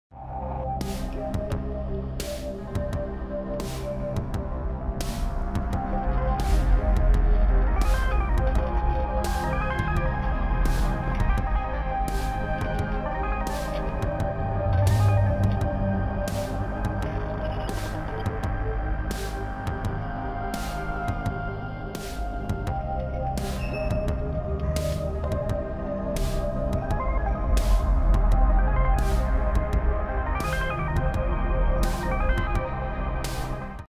Descent: Journeys In The Dark, AR Test
MR/AR Test of Fantasy Flight Games dungeon crawler, Descent: Journeys In The Dark. Technical point: the jitter between seams of walls can easily be fixed.